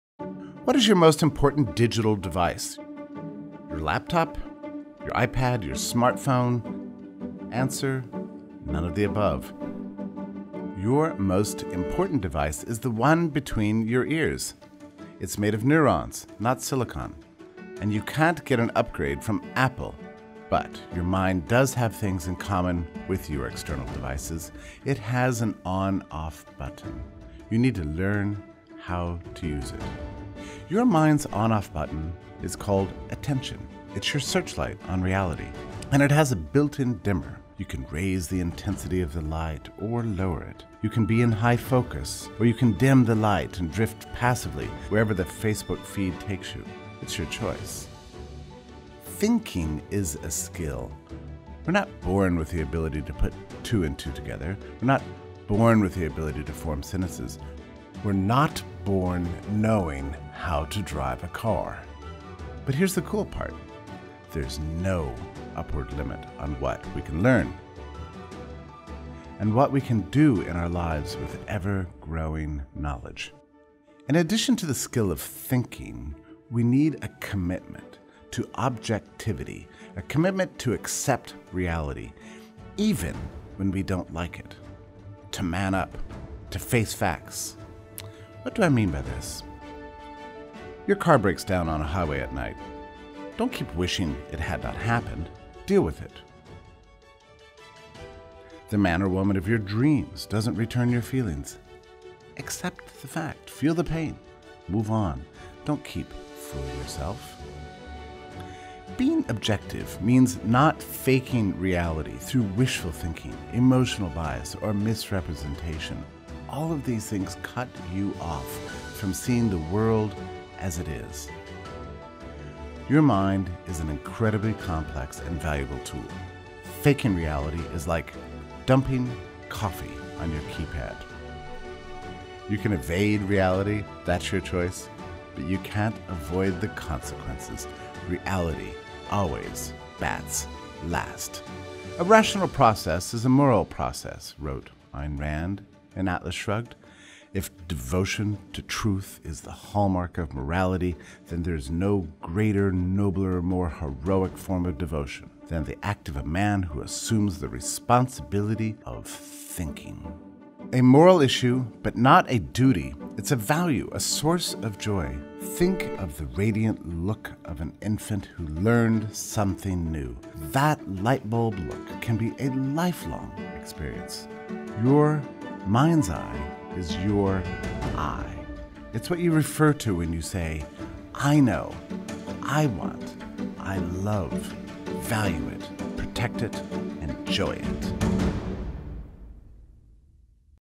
What is Reason?
Final of the four videos created by The Atlas Society for the SFL Academy course: Introduction to Objectivism